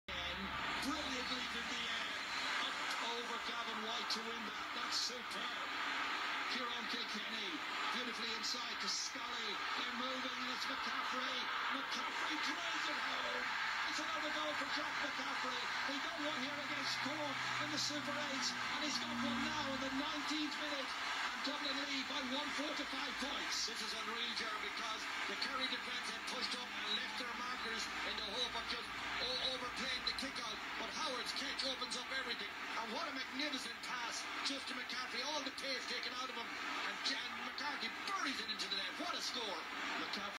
Dublin Scores All Ireland Final 2019 (Draw) Goal Jack McCaffery
Dublin’s Goal